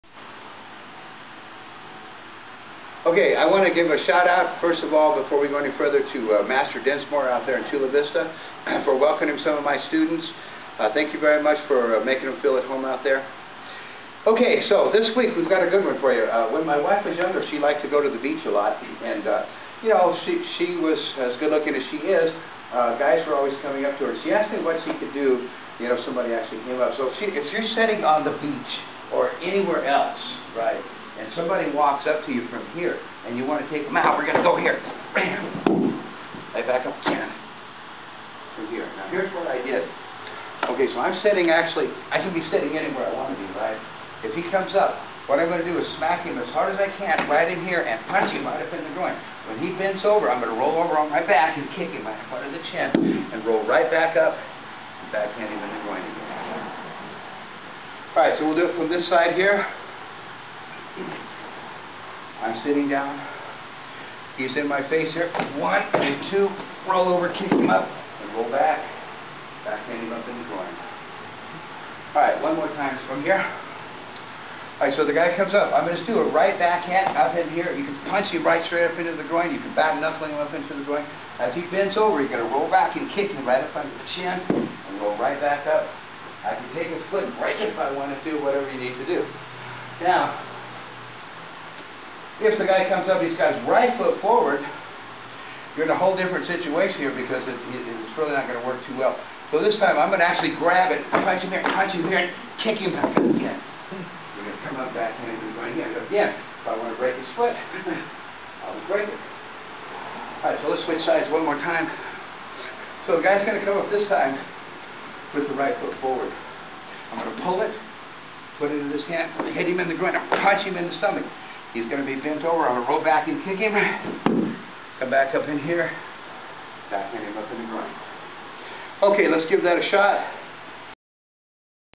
Kung Fu San Soo Yellow Belt technique of the week taught 9-8-11
Master Bill Hulsey teaching a young belt Kung Fu San Soo technique.  Visit sansoo.com and our store.  The Choy-Li-Ho Picture Lesson Book  #1-50 is now available.  These techniques are real and dangerous and only should be practiced under the guidance of a professional instructor.